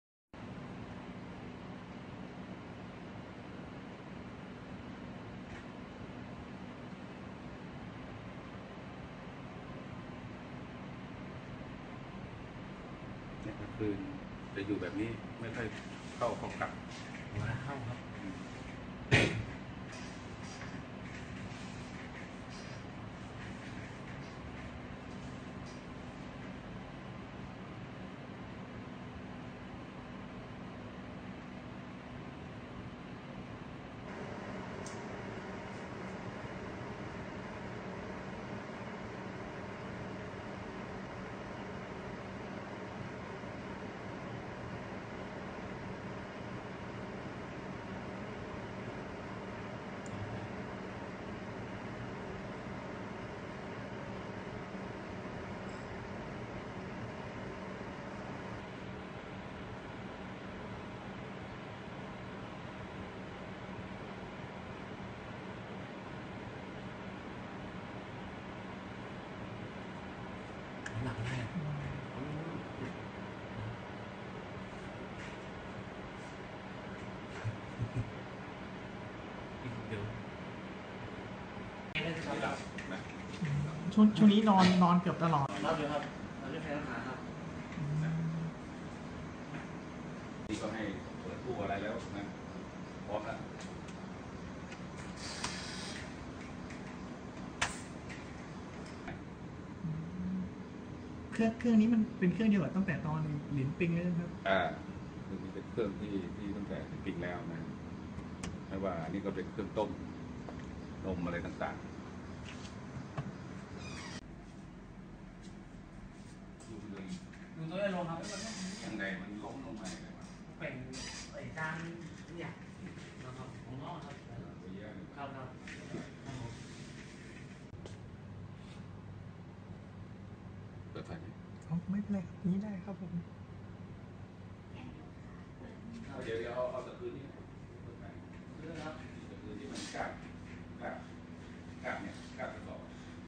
“หลินฮุ่ย” ยังดื้อไม่ยอมให้อัลตร้าซาวด์ตรวจท้อง-ฮอร์โมนพุ่งซ้ำสั่งเตรียมพร้อม
2017/05/22 หลินฮุ่ย” ยังดื้อไม่ยอมให้อัลตร้าซาวด์ตรวจท้อง-ฮอร์โมนพุ่งซ้ำสั่งเตรียมพร้อมแผนและอุปกรณ์ลุ้นลูกหมีแพนด้าเกิด